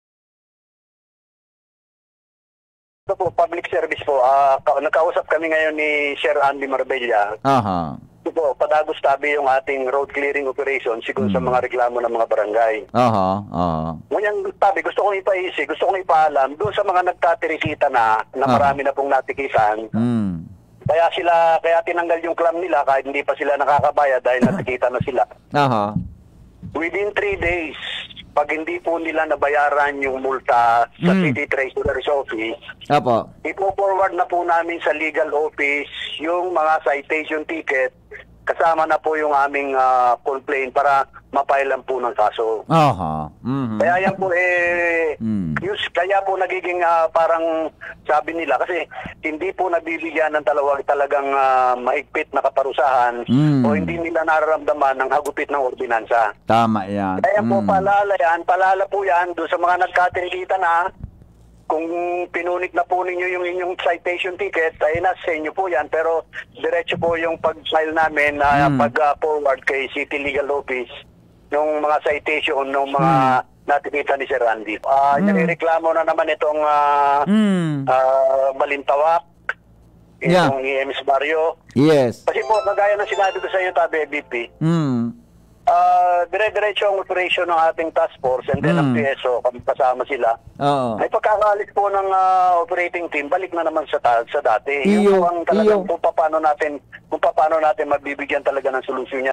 𝐏𝐒𝐎 𝐎𝐑𝐃𝐄𝐑𝐒 𝐒𝐓𝐑𝐈𝐂𝐓 𝐀𝐂𝐓𝐈𝐎𝐍 𝐀𝐆𝐀𝐈𝐍𝐒𝐓 𝐈𝐋𝐋𝐄𝐆𝐀𝐋 𝐏𝐀𝐑𝐊𝐈𝐍𝐆
Padagos an road clearing kan PSO asin Legazpi City Task Force Kalinisan at Kaayusan segun sa mga reklamo kan mga barangay.
Ini an sinabi ni Public Safety Office Chief, Ret. Col. Rolly Esguerra sa intrivista kan DZGB News kun saen saiyang ipaisi sa mga na ticket-an na sa laog nin 3 aldaw na dae pa nababayadan an saindang multa sa treasurer office, ifo-forward ninda an nasabing citation ticket asin complaint sa legal office para mai-file nin kaso.

Katuyuhan kan ordinansa na matawan nin mahigot na penalidad asin madisiplina an mga motorista.

Tune in for more updates